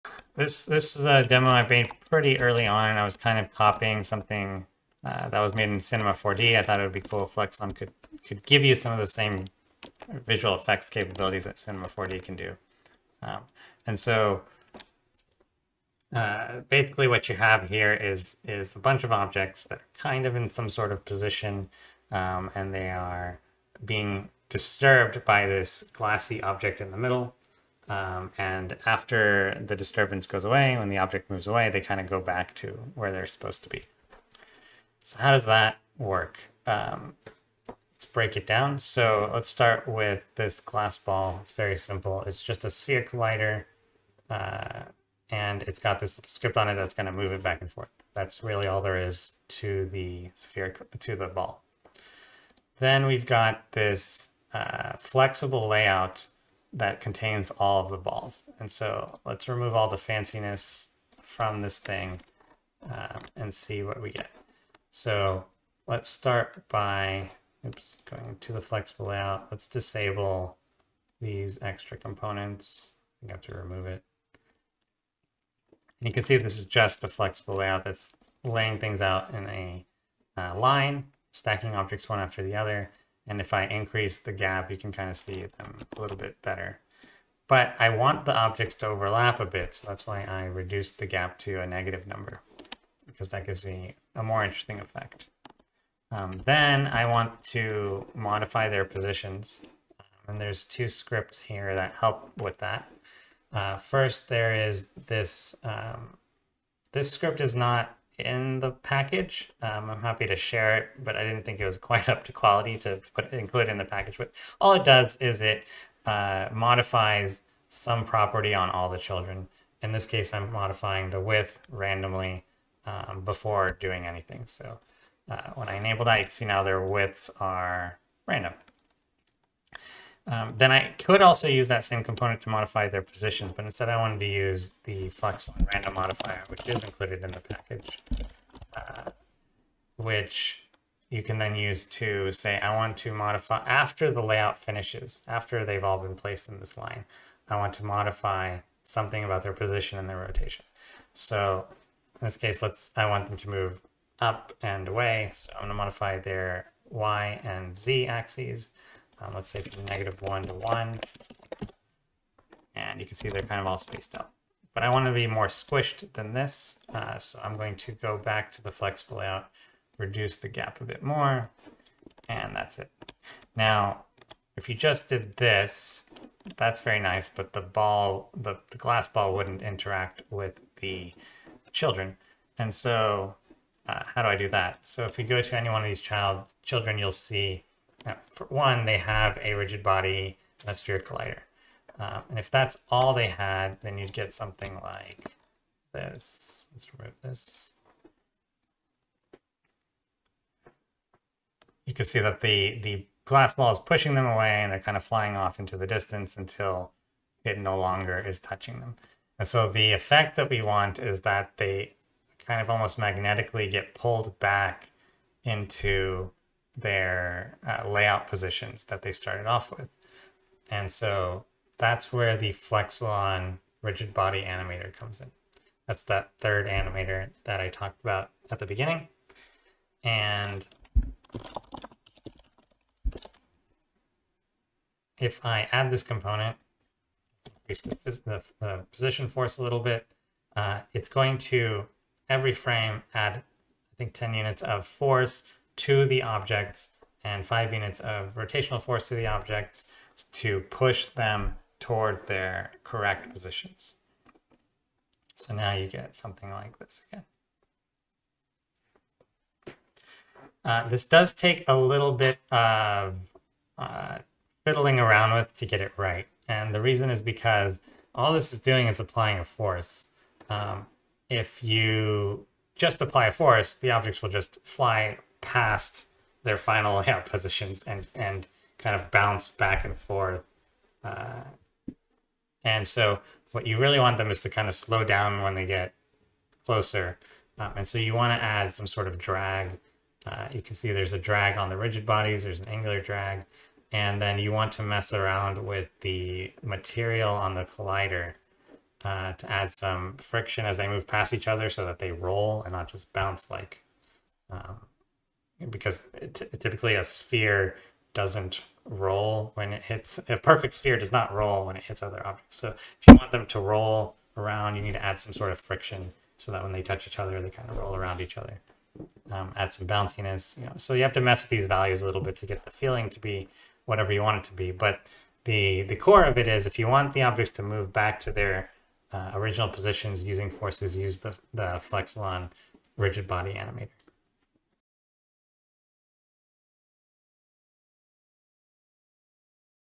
Make this cool physics effects in Unity | Flexalon
Flexalon 3D Layouts gives level designers, animators, and interface designers the power to make adaptive 3D layouts that are precise and easy to update.